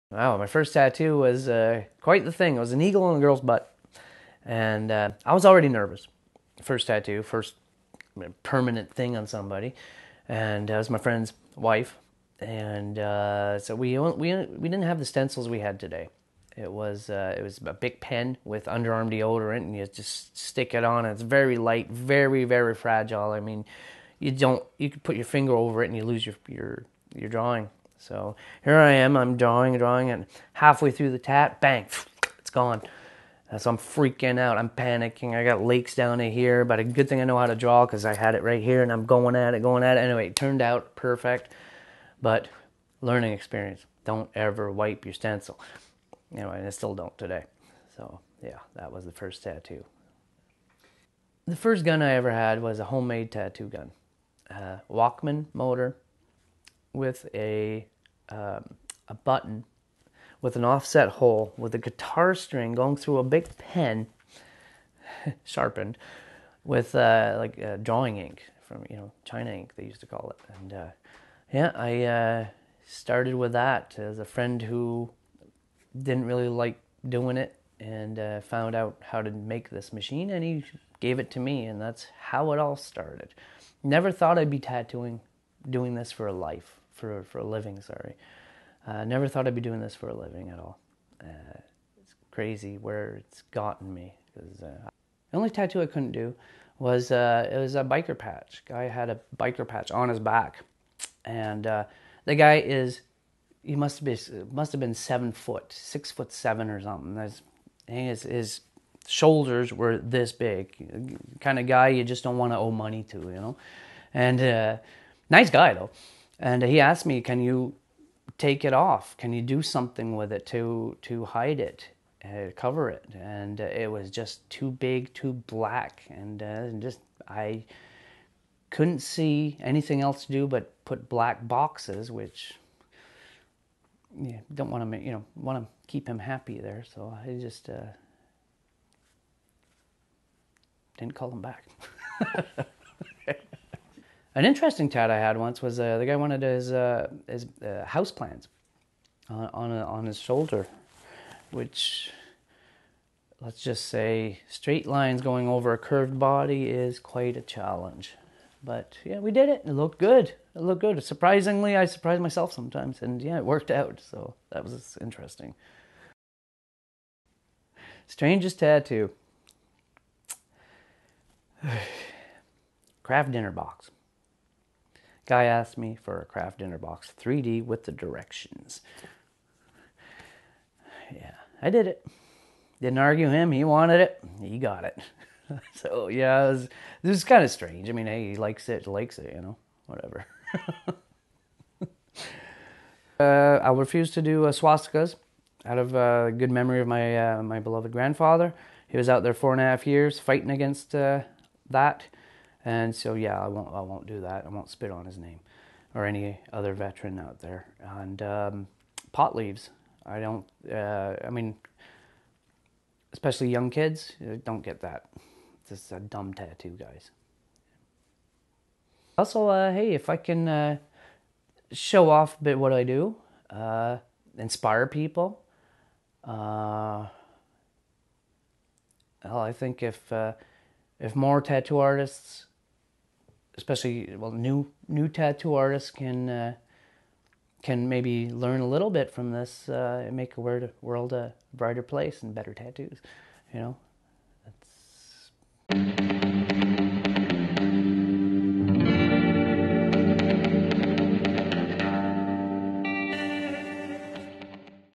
The Parlour - Mike's Thoughts on Tattoos
Mike shares some thoughts about tattoo guns, his first tattoo and how he got into tattooing.